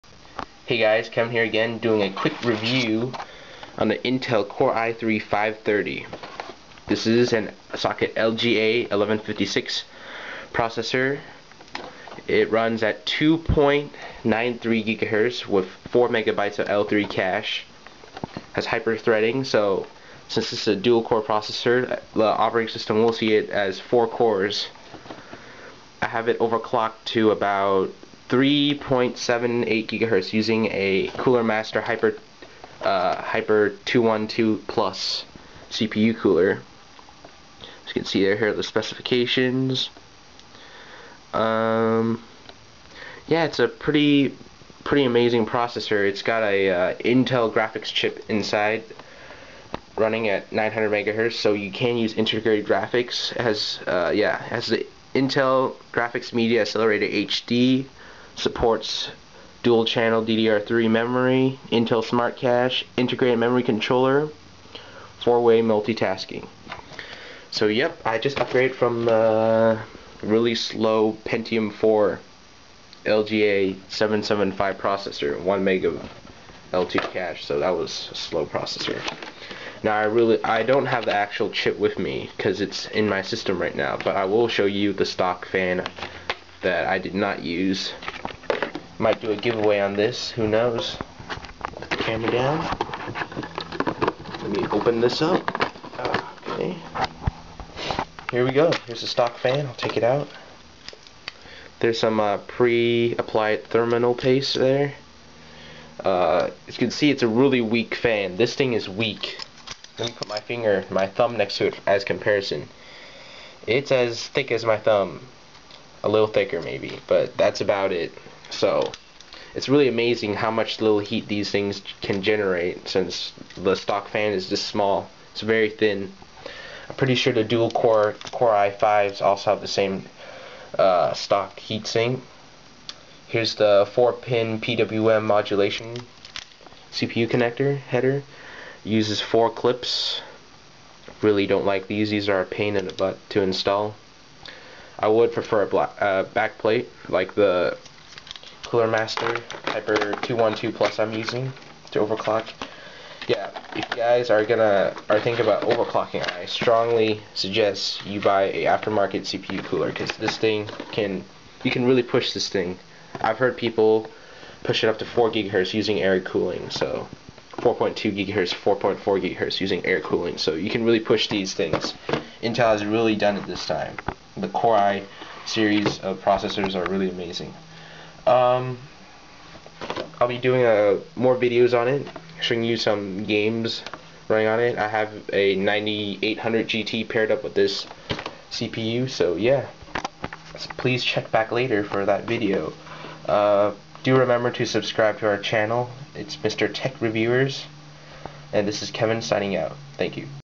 Intel Core i3 530 Review
I review on the Intel Core i3 530 CPU. This is the cpu I'm using inside my studio pc =)  I don't have the chip here since it is currently in my pc. But I do have the cooler, not a very thick cooler, but should work fine if you are running at stock speeds. I overclocked this thing to 3.74ghz, and i;m using the Cooler Master Hyper 212+. 4mb of L3 cache, Dual Core DDR3 dual channel support LGA 1156, 2.93ghz clock, hyper threading, 73 watt TDP, 32nm clarkdale, integrated Intel HD graphics chip!